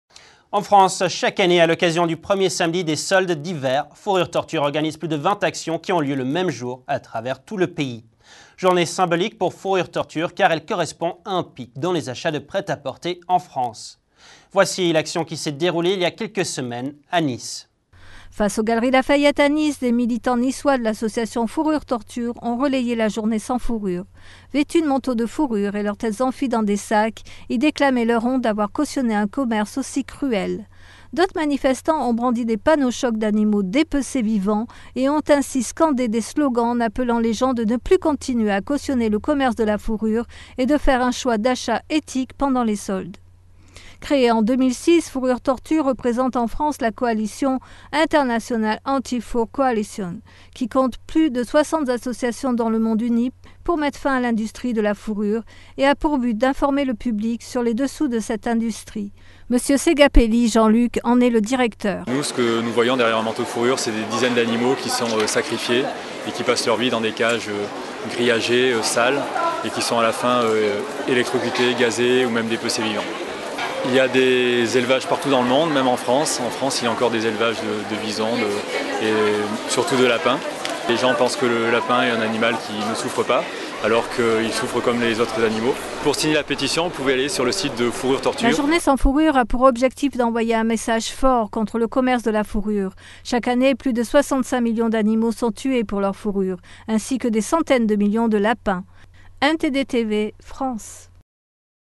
Les militants de Fourrure torture manifestent
Chaque année à loccasion du premier samedi des soldes dhiver qui débute le 9 janvier, Fourrure Torture organise plus de vingt actions qui ont lieu le même jour à travers toute la France Journée symbolique pour Fourrure Torture car elle correspond à un pic dans les achats de prêt-à-porter en France